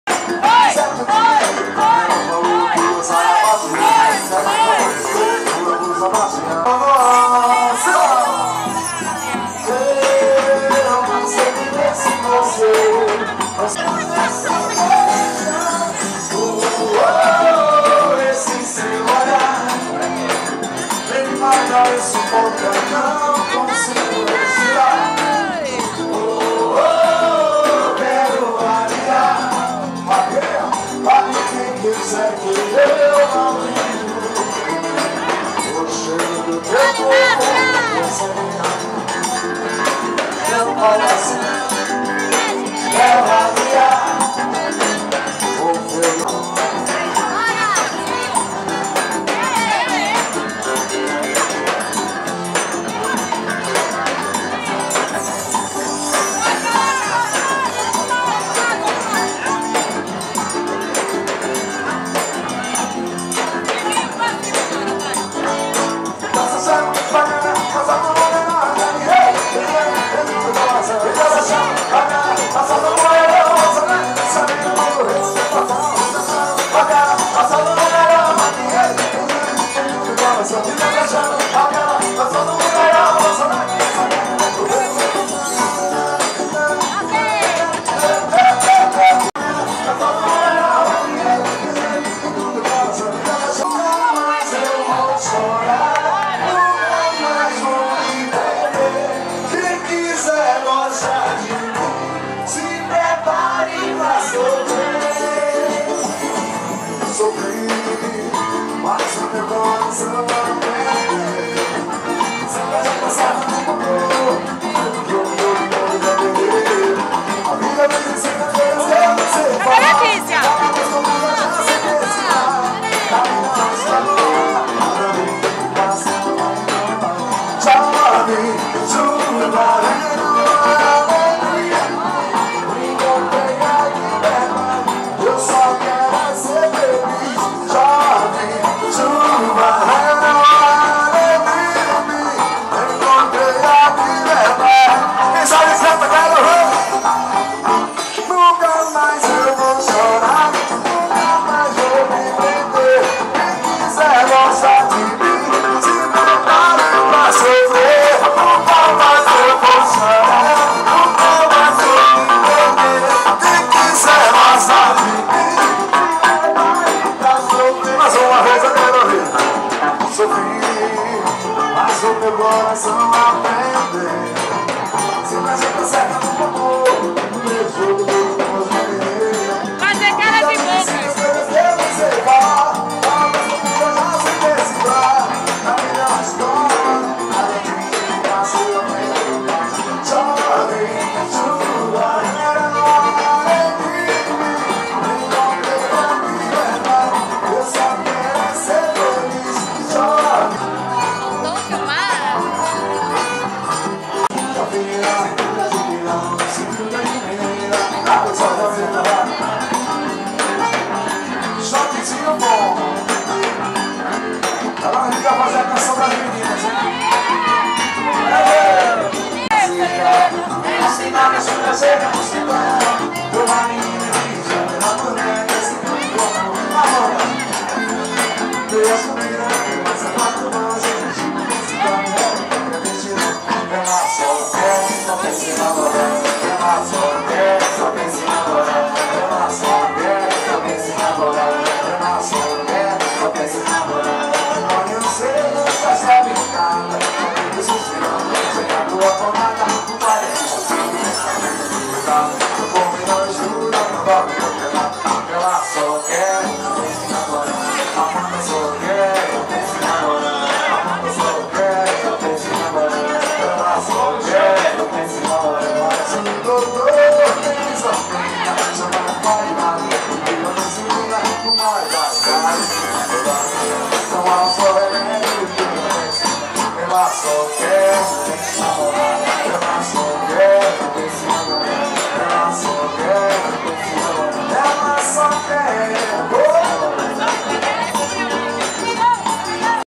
Forró da A Vaidosa/2011 (Vídeo 1/3)
Festa Junina da Troça carnavalesca A Vaidosa, realizada em 19/06/2011, no Clube Cariri de Olinda - PE.